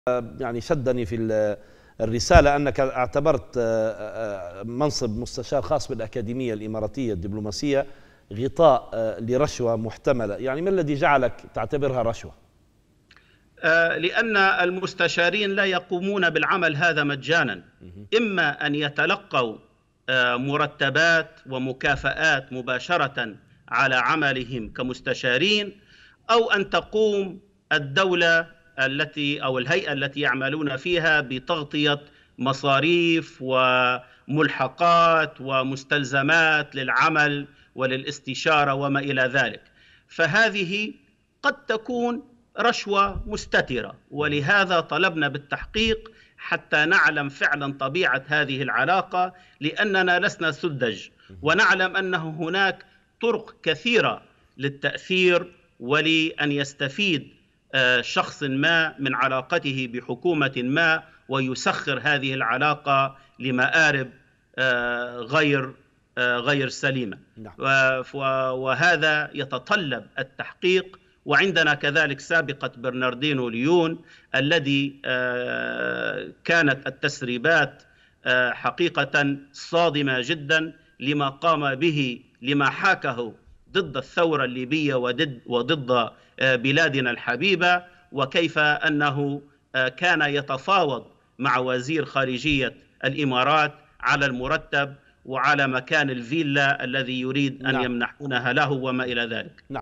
لماذا يعتبر كون سلامة مستشاراً لأكاديمية إماراتية محل شكوى؟ | أ. عمادالدين المنتصر
من تغطية خاصة | أ. عمادالدين المنتصر | رئيس مؤسسة الديمقراطية وحقوق الإنسان | لماذا يعتبر كون سلامة مستشاراً لأكاديمية إماراتية محل شكوى؟ | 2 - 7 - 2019

ــــــــــــــــــــــــــــــــــــــــــــــــــــــــــــــــــــــــــ
ــــــــــــــــــــــــــــــــــــــــــــــــــــــــــــــــــــــــــ
يمكنكم تحميل تطبيقنا TANASUH TV على الهواتف الذكية: